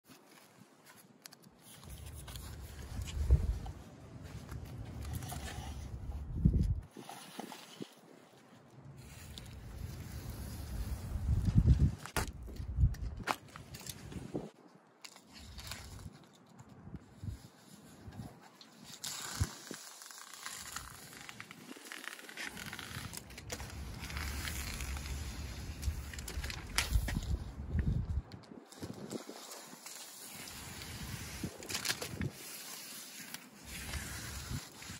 Game Changing Window Tape
Fentrim tape from SIGA is a total
Game changer!!  Anytime you install windows with mail fins that aren’t 100% molded into the frame, there’s a good chance you want to make sure your seal tape laps over the frame.   This can be a huge pain with regular tapes.  With the fentrim that includes a built in 1/2” fold, it is so easy anyone can do it!   Even better yet this tape is amazing over and has great attributes.